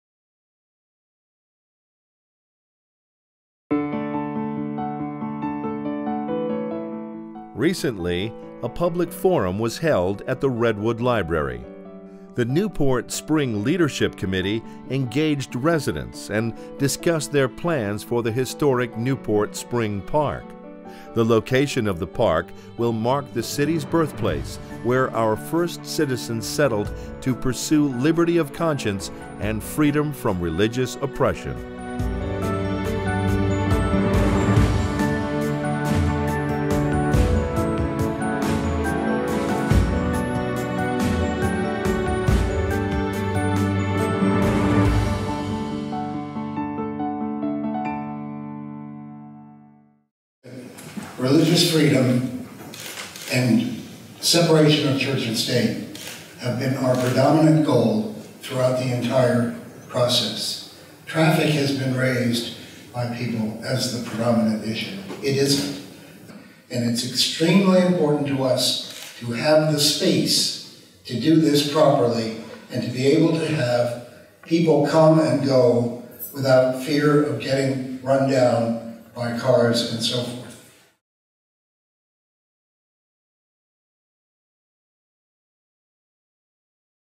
Historic Newport Town Spring Park Forum 3.22.2019
Please enjoy clips from the March 22, 2019, public forum at the Redwood Library in which the Historic Newport Town Spring Park and its connection to religious liberty were discussed.